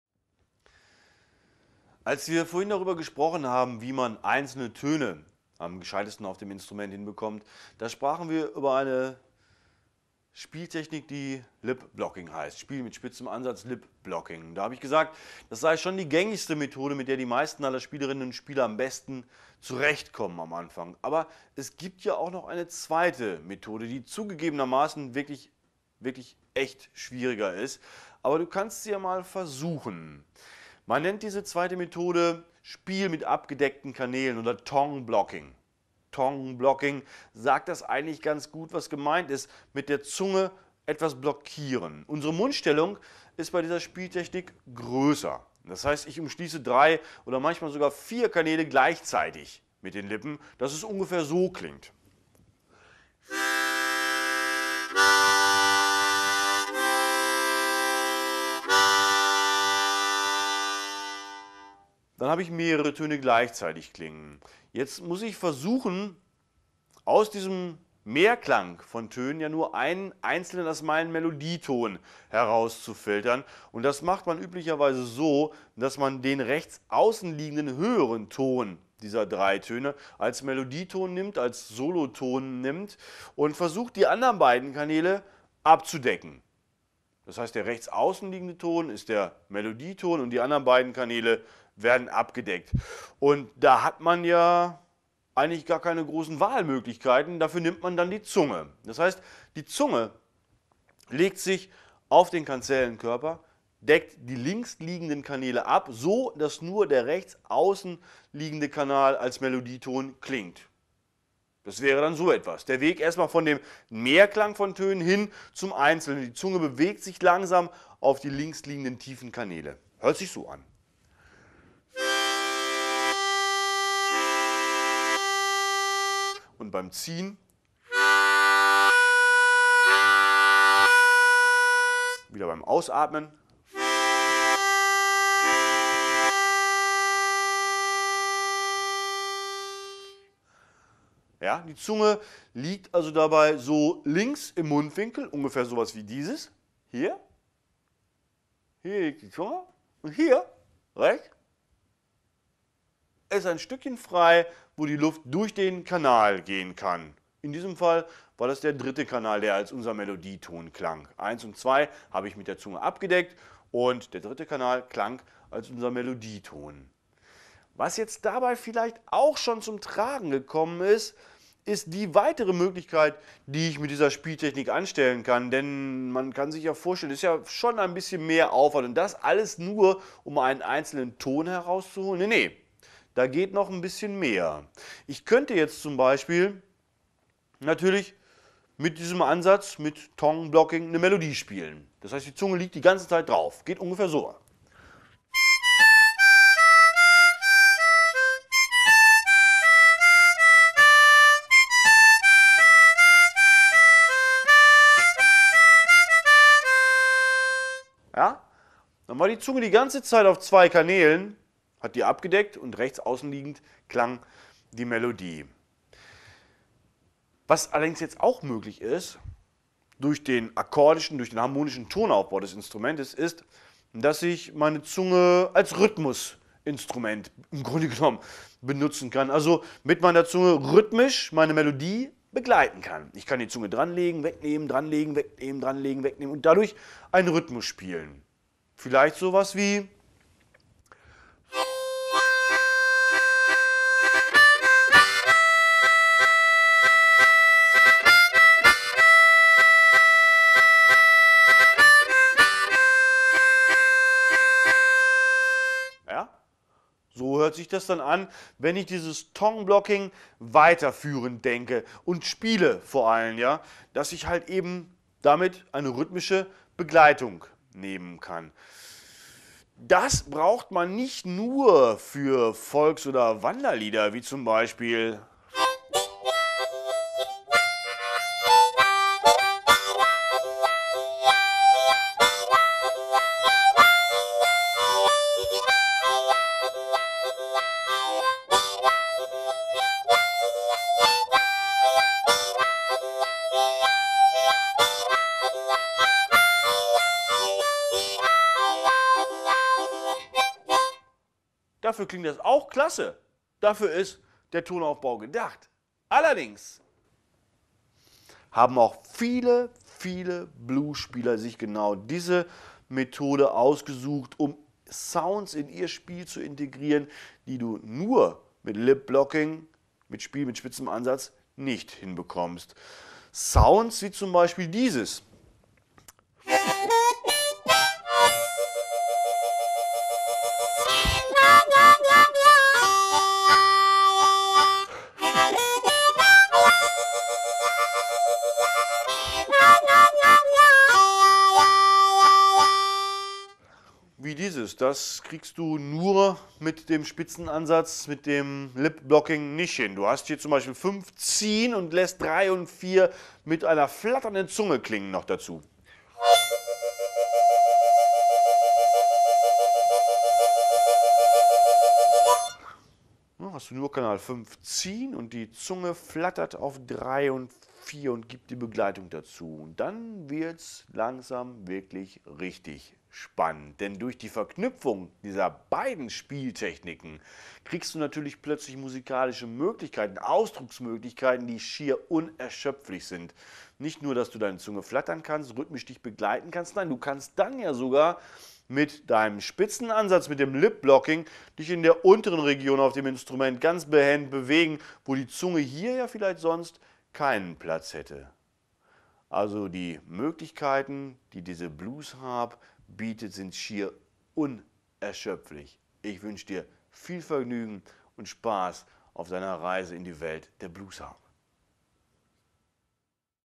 "Blues Harp Schule, für Einsteiger" - DVD Track 8. - tongue blocking / Spiel mit abgedeckten Kanälen
Blues Harp Schule, für Einsteiger" (Voggenreiter Verlag) - DVD Track 8, 6'53"

- tongue blocking / Spiel mit abgeckten Kanälen

: Die Technik

: Die Einsatzmöglichkeiten

: C-Harp, C-Dur / G-Dur